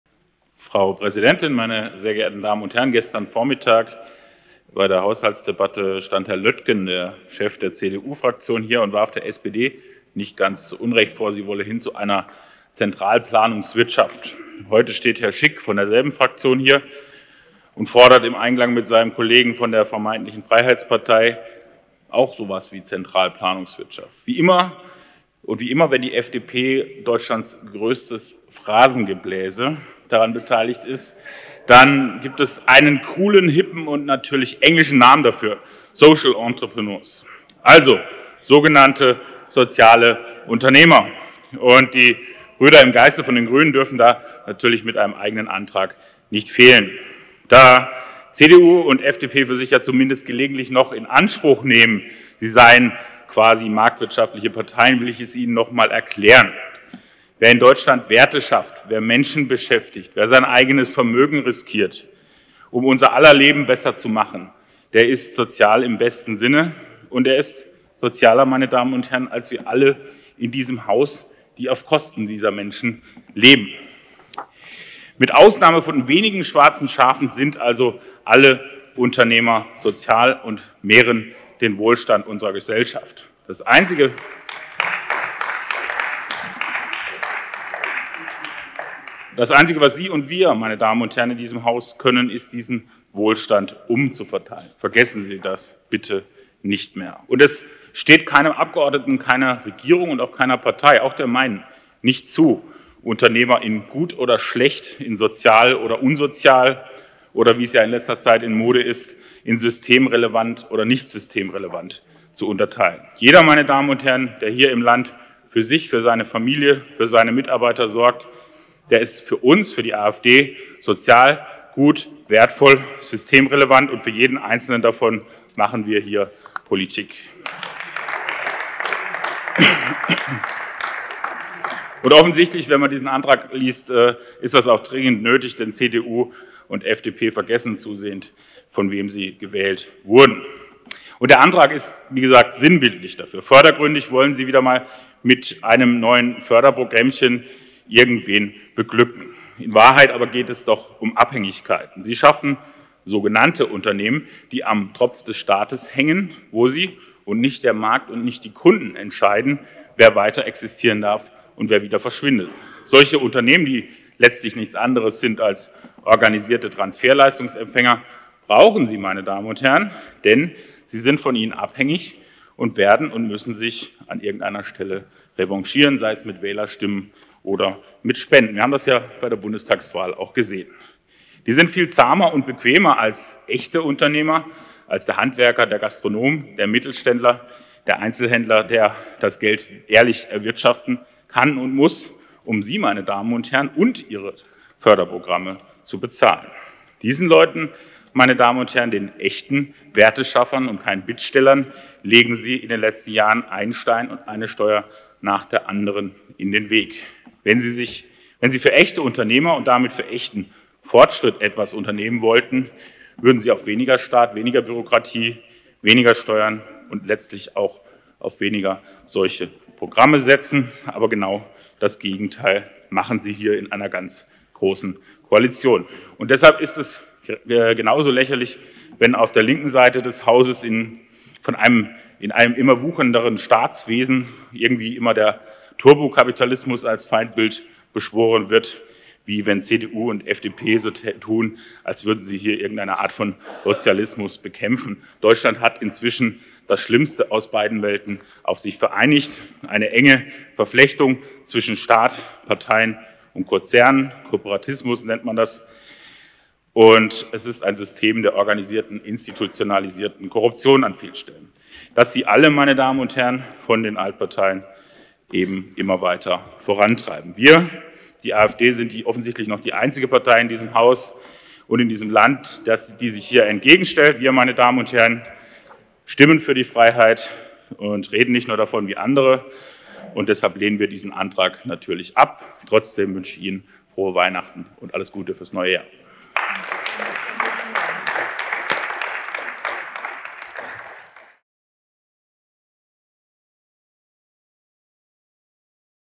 AfD vs FDP! – Sven Tritschler (AfD)
Keine Fraktion hat ihr Wahlprogramm derart schamlos verraten wie die FDP. Sven Tritschler (AfD-Fraktion NRW) rechnet mit den sogenannten Liberalen ab und erteilt ihnen eine saftige Lektion.

Rede gehalten am 16. Dezember 2021 im Landtag NRW, TOP 11.

📲 Facebook.com/afdfraktionNRW/
🐥 Twitter.com/AfD_FraktionNRW